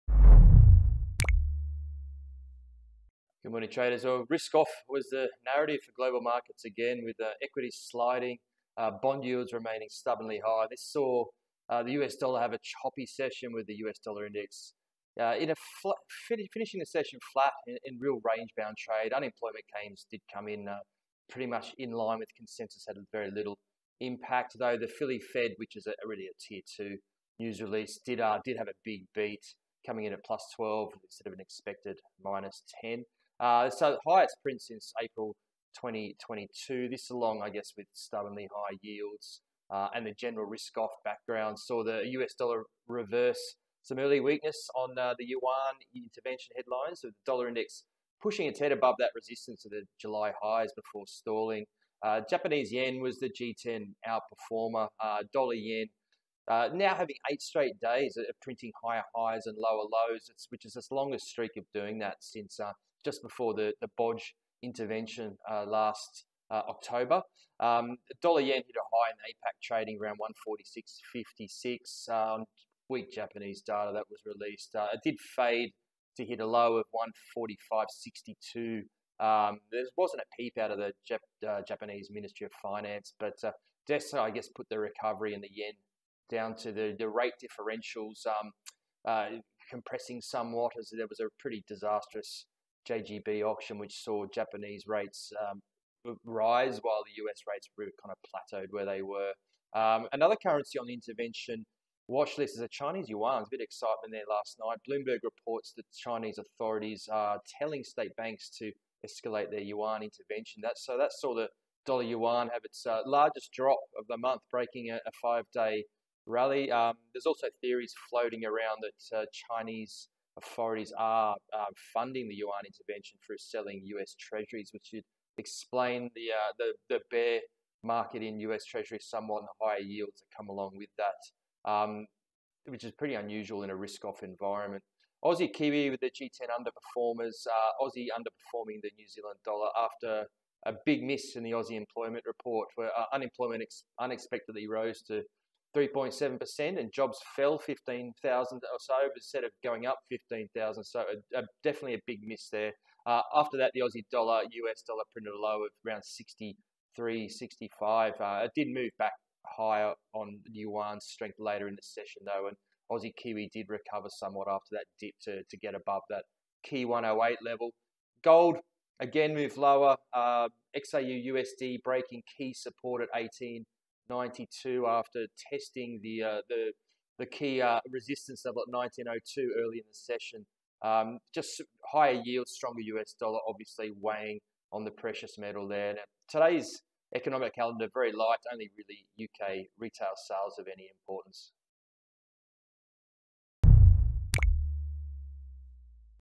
FX Analysis – CNH and JPY rally – Gold breaks key support - AUD dips on jobs report
Risk-off session in equities and stubbornly high UST yields see USD hold on in the face of CNH strength on intervention talk.
AUD dips after shocker employment report, but finds support on a re-surgent CNH